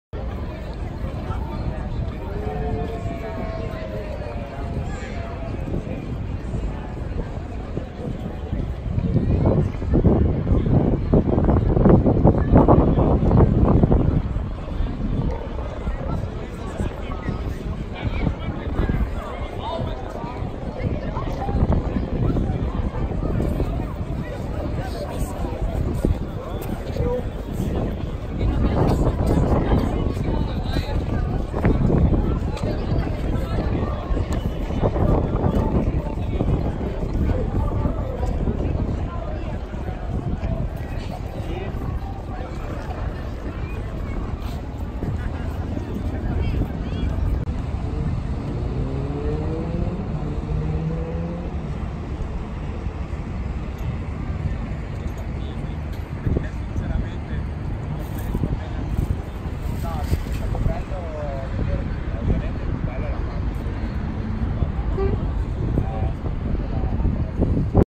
[Paris Weather Today] April 1, 2019 : Eiffel Tower, Pont Alexandre III
Paris weather Tdoay
Cloudy, 21℃ / 11℃

Enjoy Paris

1. Eiffel Tower (Trocadero)
2. Pont Alexandre III